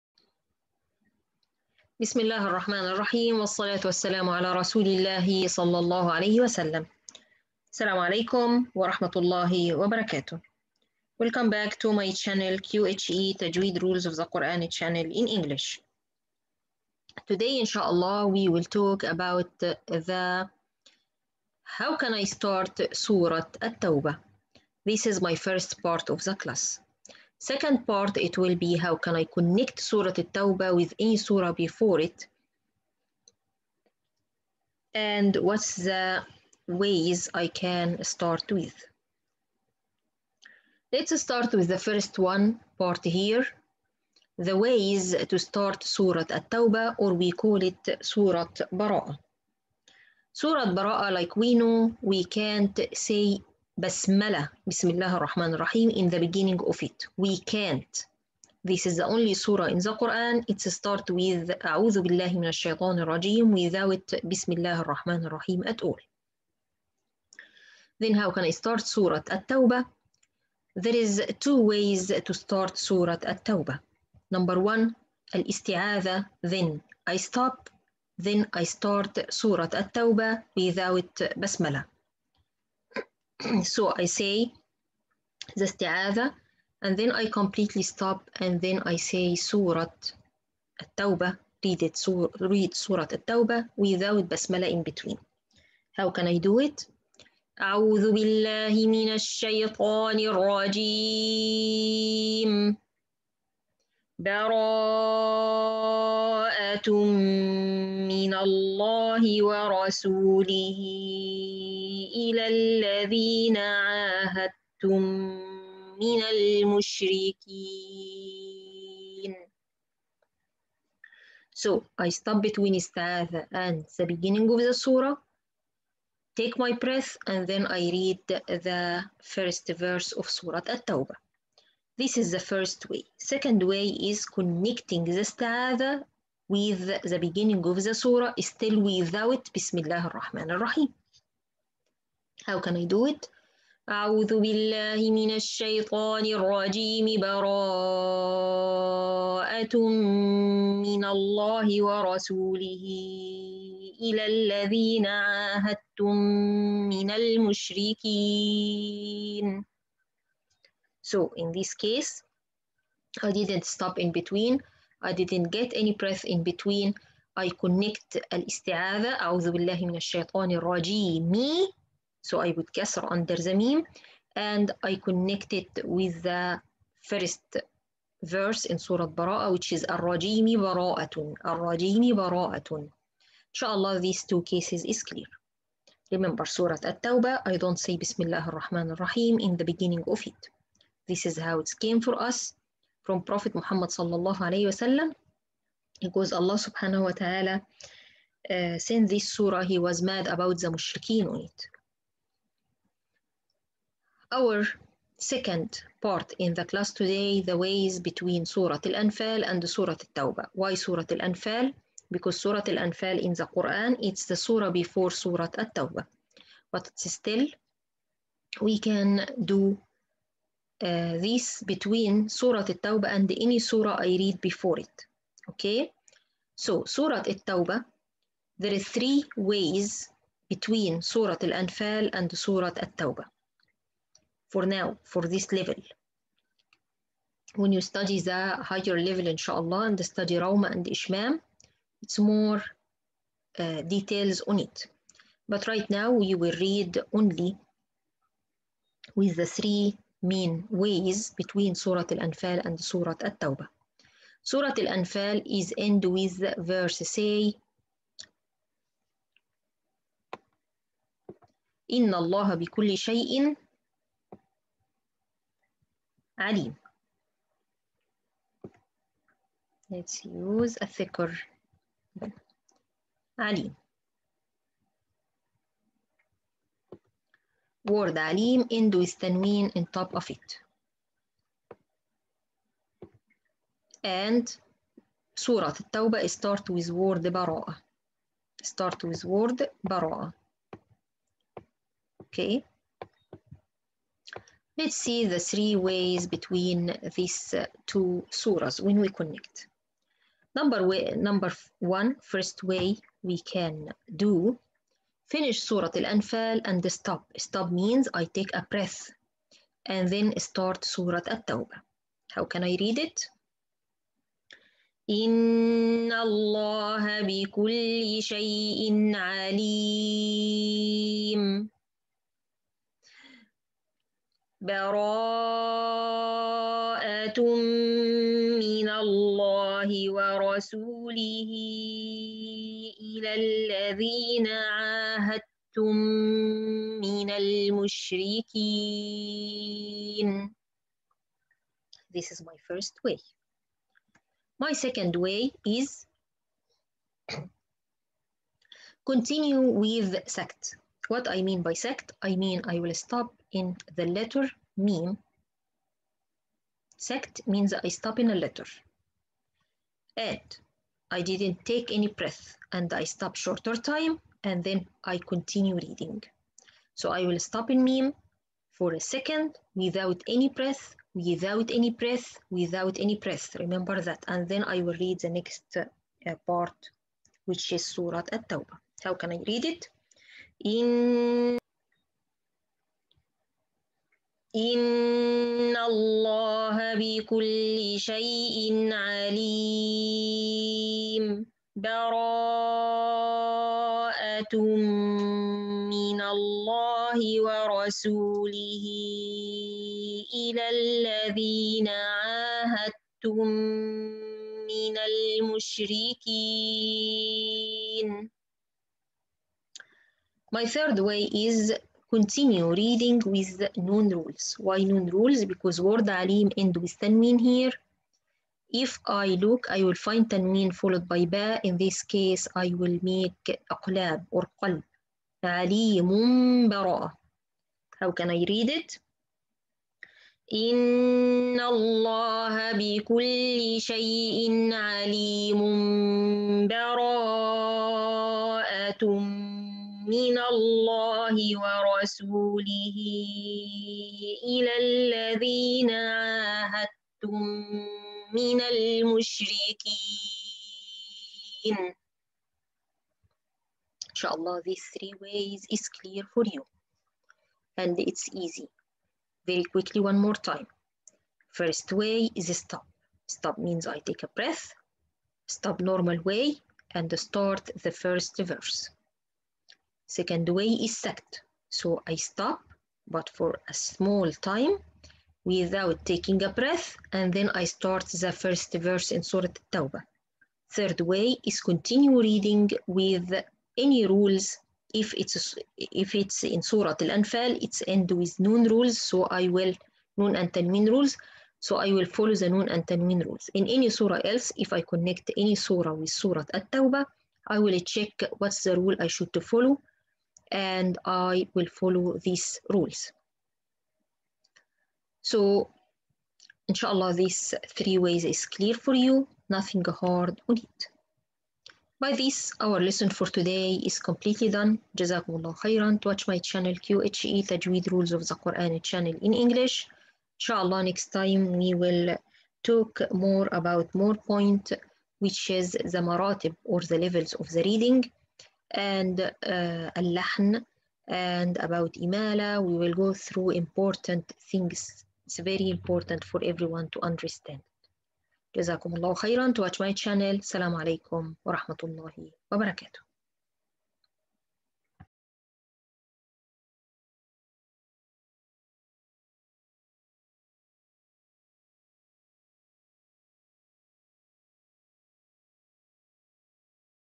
7-The ways between Surah Al-Anfal and Surah At-Tawba
The ways to start surat At-Tawba  كيفية البدء بسورة التوبة
The ways between Surah Al-Anfal and Surah At-Tawba كيفية وصل سورة الانفال بسورة التوبة
How can I connect any surah with surah At-Tawba
كيفية وصل اي سورة بسورة التوبة
وهذا لينك اليوتيوب and this is the YouTube link☝️☝️☝️